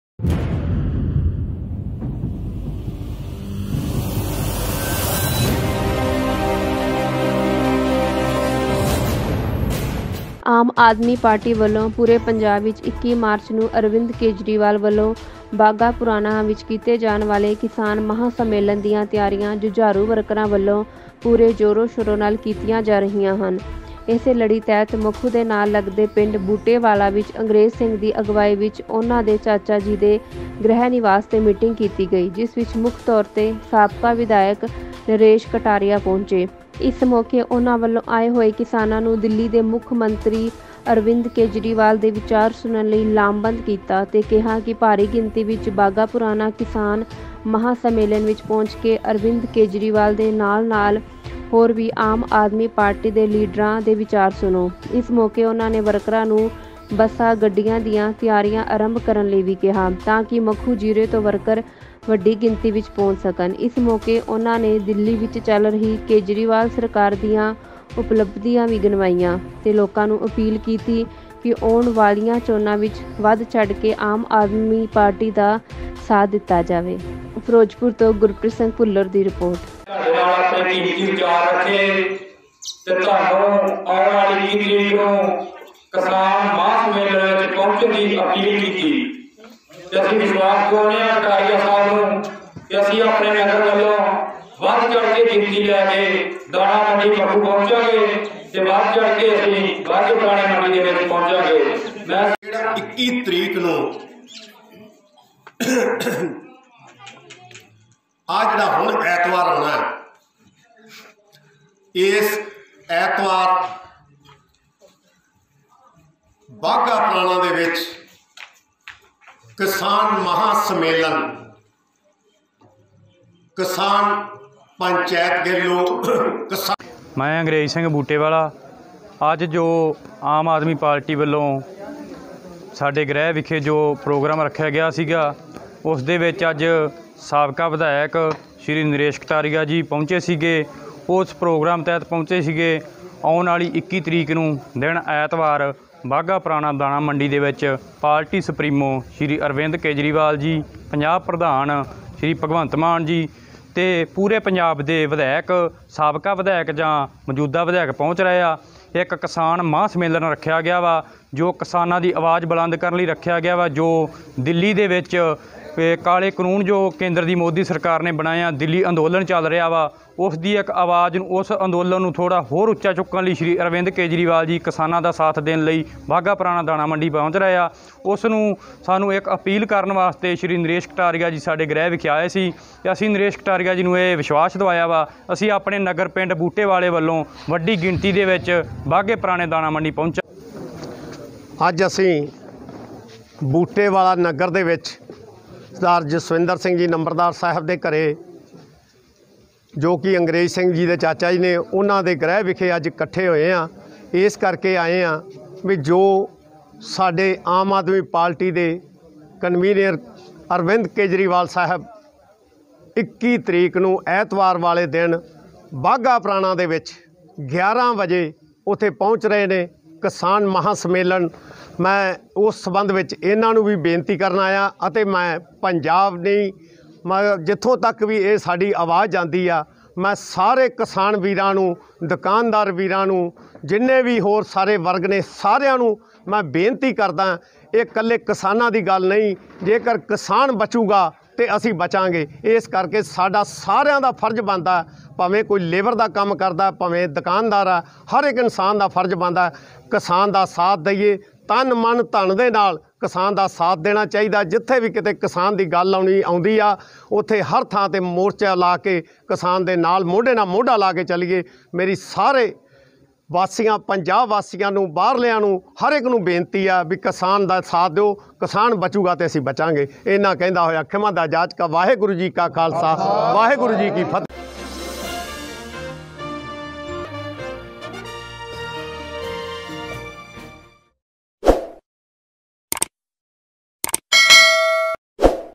21 ਮਾਰਚ ਦੀਆਂ ਤਿਆਰੀਆਂ ਨੂੰ ਲੈ ਕੇ ਪਿੰਡ ਬੂਟੇ ਵਾਲਾ ਵਿਚ ਅੰਗਰੇਜ਼ ਸਿੰਘ ਦੀ ਅਗਵਾਈ ਵਿੱਚ ਕੀਤੀ ਗਈ ਮੀਟਿੰਗ
21 ਮਾਰਚ ਦੀਆਂ ਤਿਆਰੀਆਂ ਨੂੰ ਲੈ ਕੇ ਪਿੰਡ ਬੂਟੇ ਵਾਲਾ ਵਿਚ ਅੰਗਰੇਜ਼ ਸਿੰਘ ਦੀ ਅਗਵਾਈ ਵਿੱਚ ਕੀਤੀ ਗਈ ਮੀਟਿੰਗ